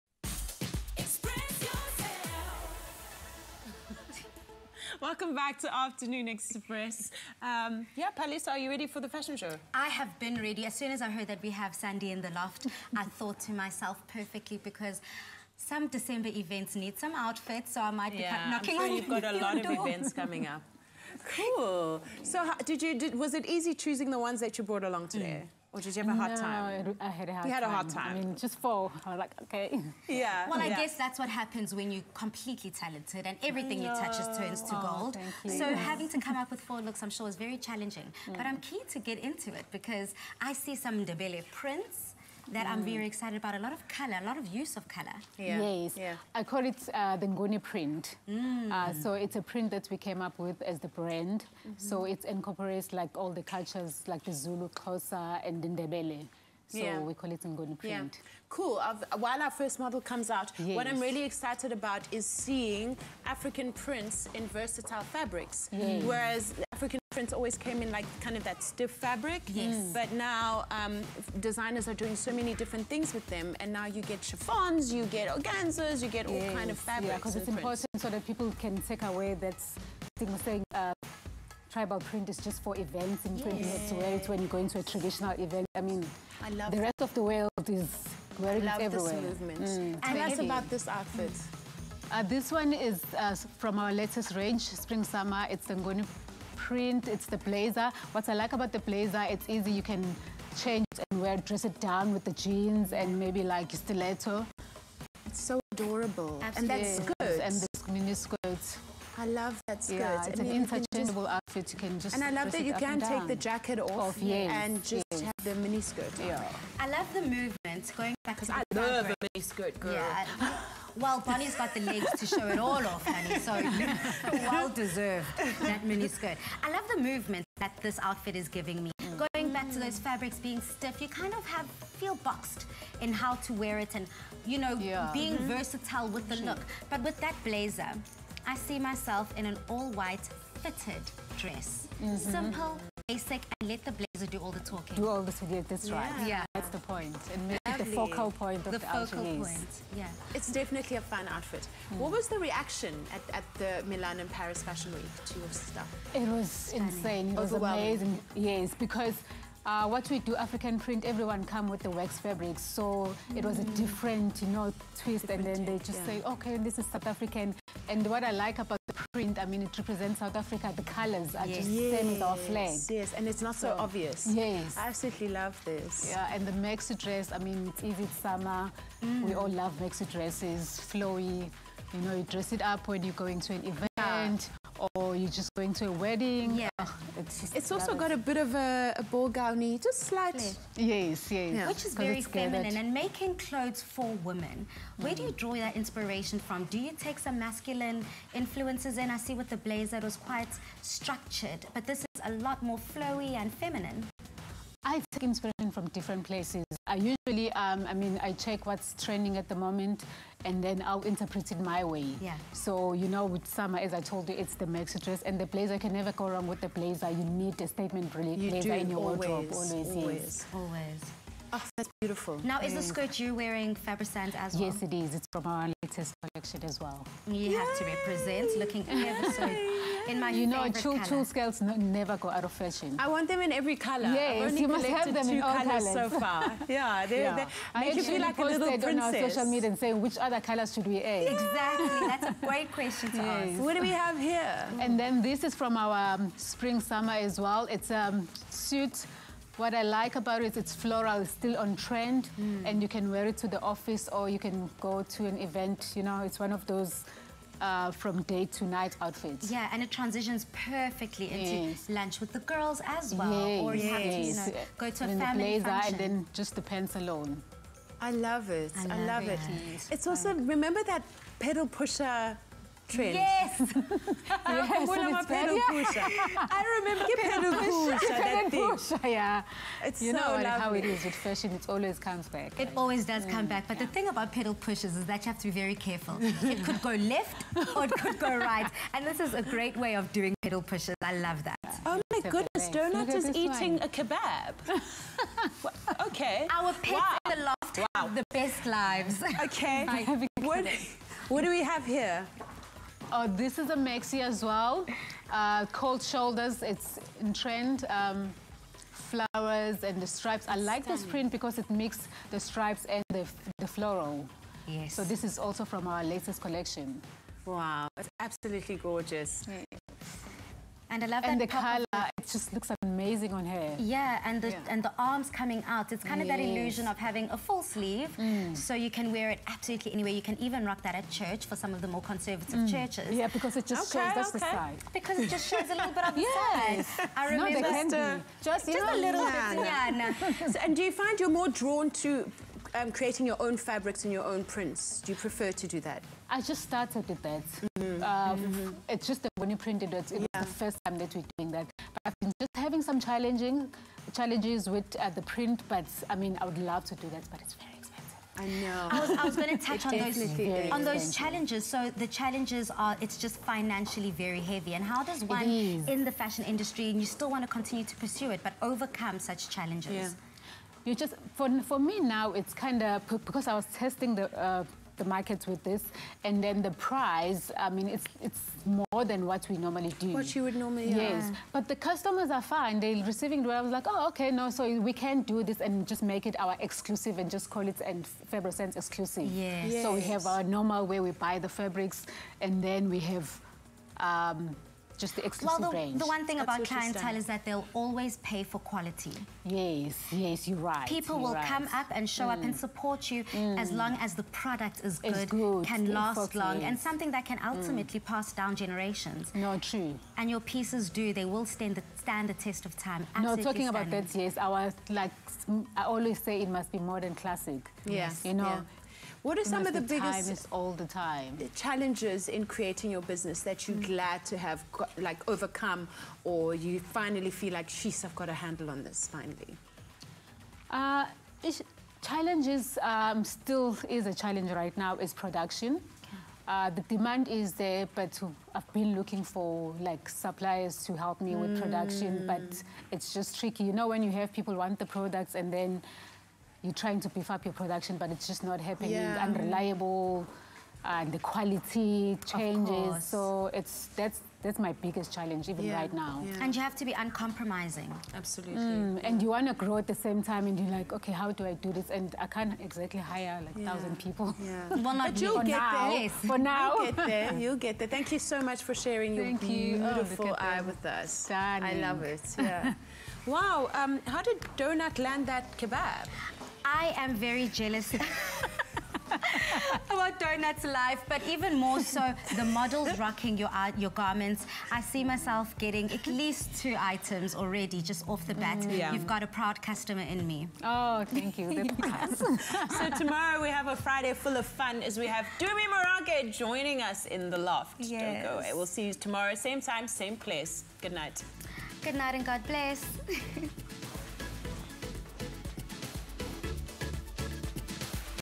Fashion Show : Sandi Mazibuko of FABROSANZ
Interview with Sandi Mazibuko, creator of the fashion brand ‘Fabrosanz’.
FabroSanz (FRS) Fabulous Royalty Sandi – “FRS” is a wholly owned African female fashion design business. Since its inception the growth of the FabroSanz (FRS) has been totally driven by FRS’s tenacity and self-drive to fulfill her vision to be a well-known and a respected brand in South Africa and Internationally. Fabrosanz Creations is a fashion brand that understands the importance of evolving in this fast paced ever growing fashion industry to continuously meet the growing needs of its clients whilst creating positive impressions with new and prospective clients. Therefore, the brand finds it imperative to explore avenues that will help the brand to focus more on being service- driven whilst offering an enhanced overall experience to suit the needs of its clientele.